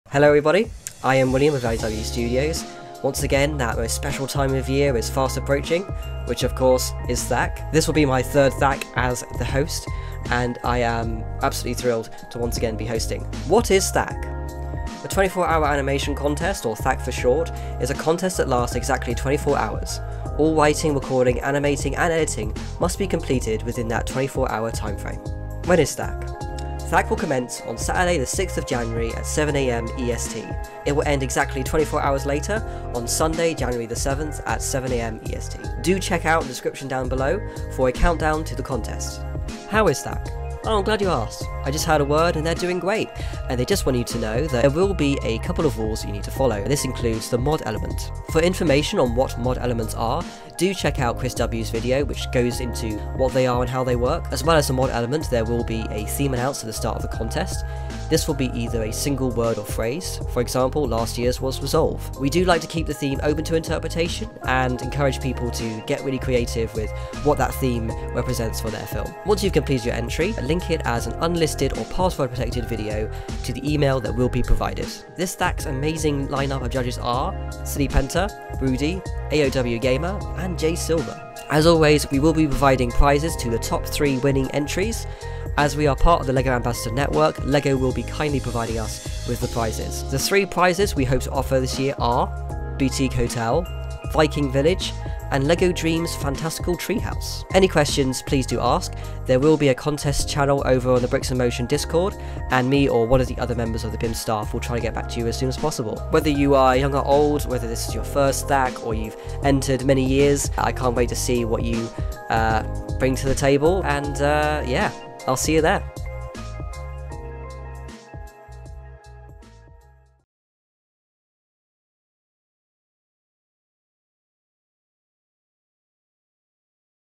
THAC 2024 Announcement (24-Hour LEGO Animation Contest)
Anyone who is not banned from Bricks in Motion is eligible to enter THAC. The email address to submit entries to will be listed in the "THAC 2024 STARTS NOW!" thread and the THAC 2024 launch video that will be posted on January 6, 2024. You do not need to have an account on Bricks in Motion, but it is encouraged that you join the site or Discord and get involved with the community. Judges may participate but are disqualified from being ranked and from receiving prizes. Joint productions are allowed, but it is up to you to divide up the prizes if you win anything.

To assure that you create your entry within the 24 hour time frame and to prevent cheating, the theme will be announced at the start of the contest. You are allowed to brainstorm ideas beforehand and/or prepare builds for your sets, but there is no guarantee that your ideas will match the theme. The theme will either be a single word or phrase. For example, the last THAC’s theme was “Resolve”.

You must include the THAC logo for at least 3 uninterrupted seconds of your entry. This could be at any point in your video and any variation of the logo that will be provided. Several high-quality resolution versions of the THAC 2024 logo can be found here:

 / bricksinmotion

Transcript